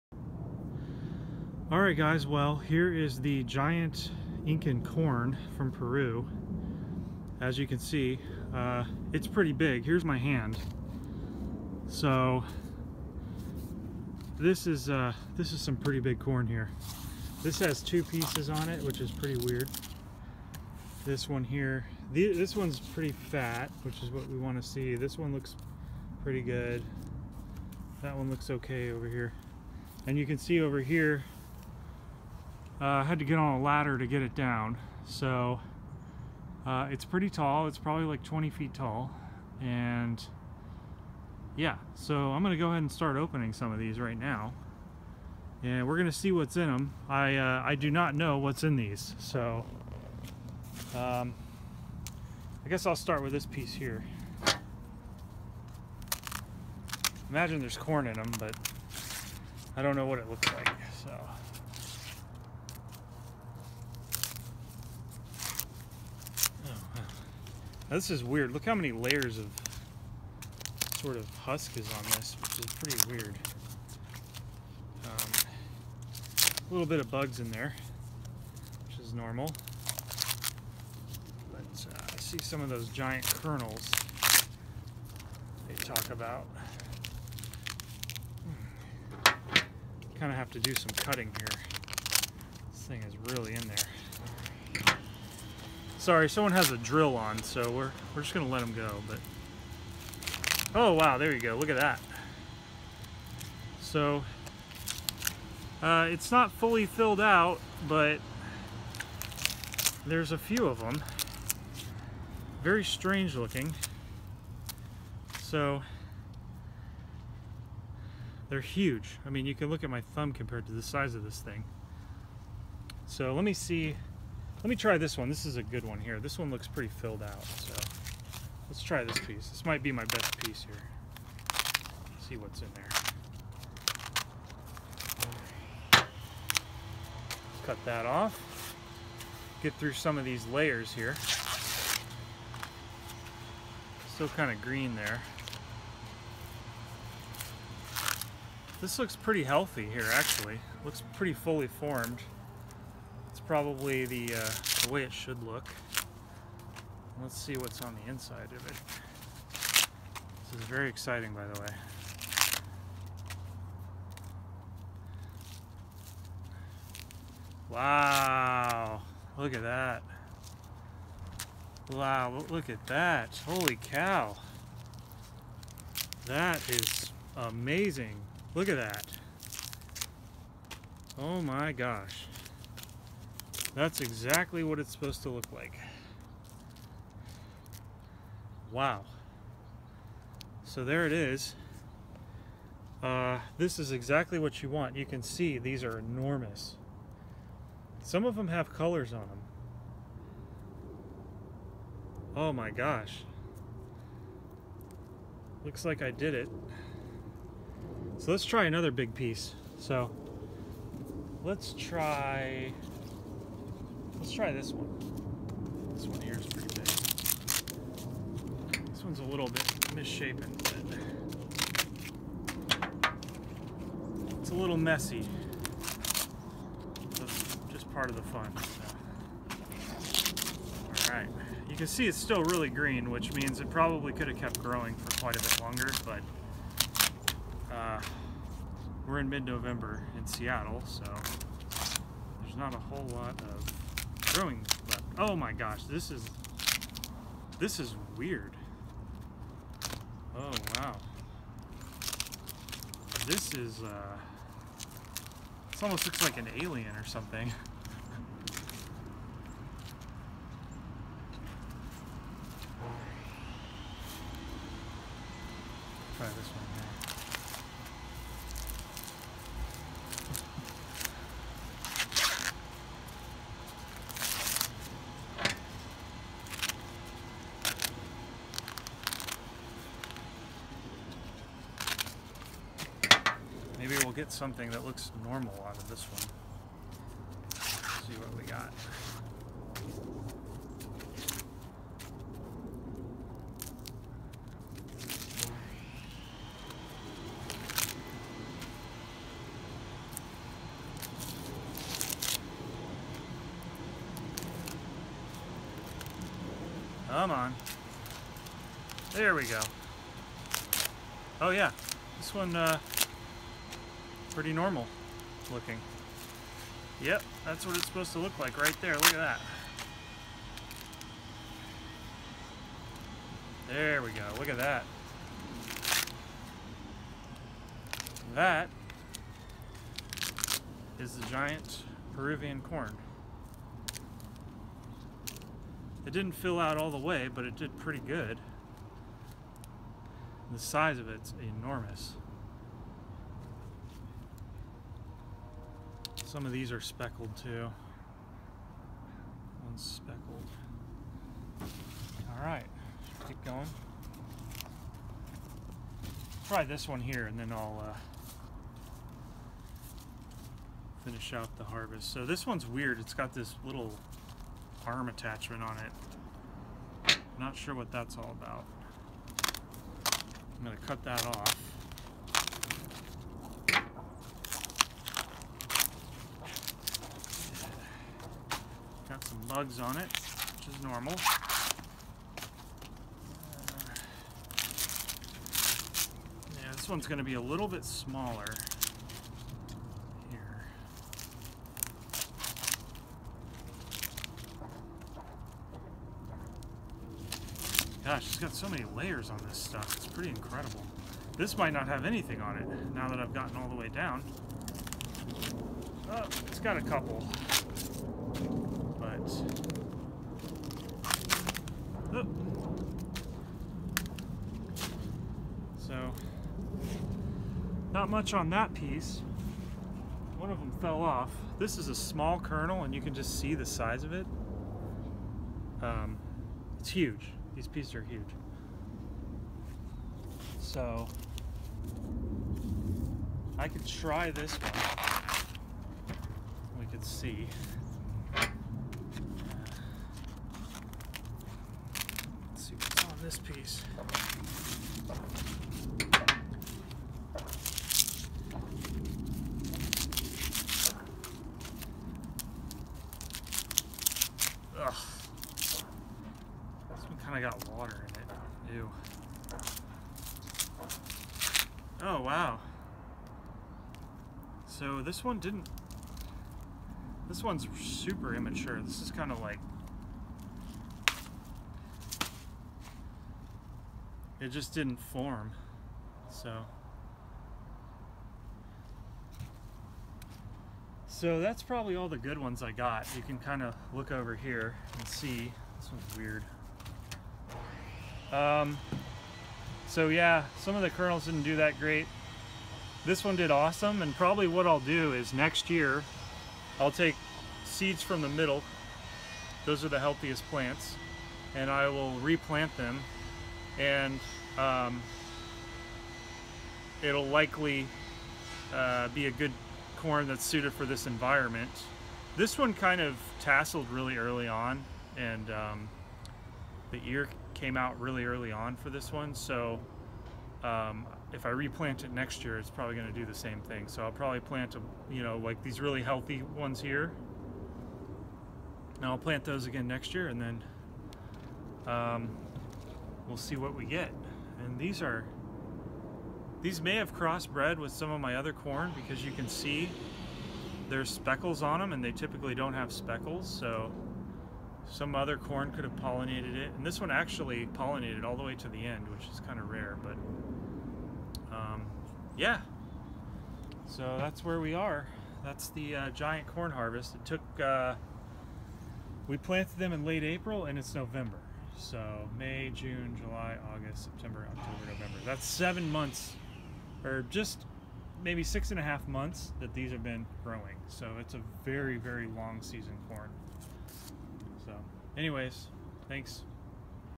Giant Corn opening, Peruvian corn choklo corn harvest
Finally opening the giant corn from Peru after growing from April until November. Almost a 7 month growing season and 16 plants!! I tended to these very carefully. I watered every day and fertilized them 4 different times with a high nitrogen mix. The climate here is somewhat similar to the Peruvian Andes except we get alot longer days in summer. Obviously our winters are much colder but the growing season temperature and humidity is almost the same. The longer days may have thrown off some of the plants. Regardless I still got a couple of pieces that looked normal and I will be taking the largest and healthiest seeds to plant for next year. This was an awesome project that took several months and I did everything to make this corn happy. I think it paid off. Sad to see my giant corn grow up but we have new ones next year! Really had fun with this!!